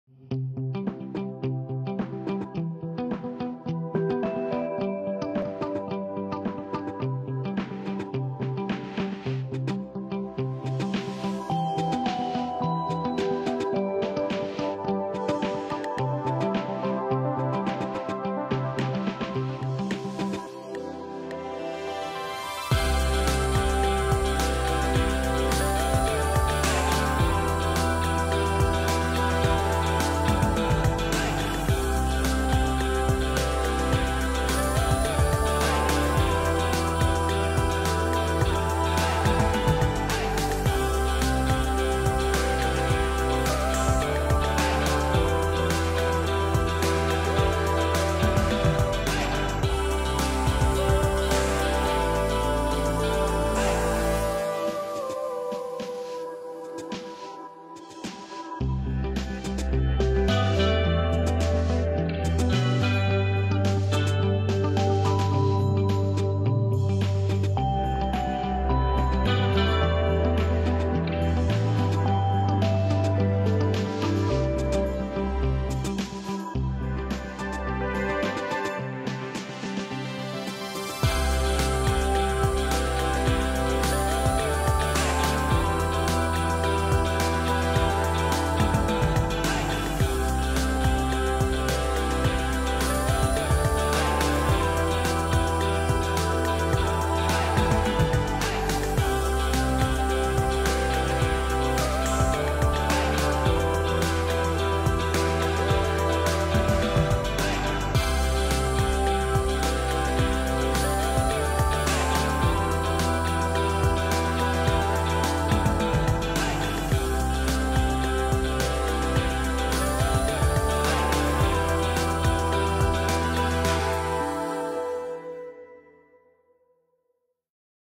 Full Body Resistance Band Workout | Total Body Conditioning
This full body resistance band workout will tone and strengthen the body. 30 seconds on each exercise with 10 seconds recovery; 4-rounds with 45 seconds rest between rounds.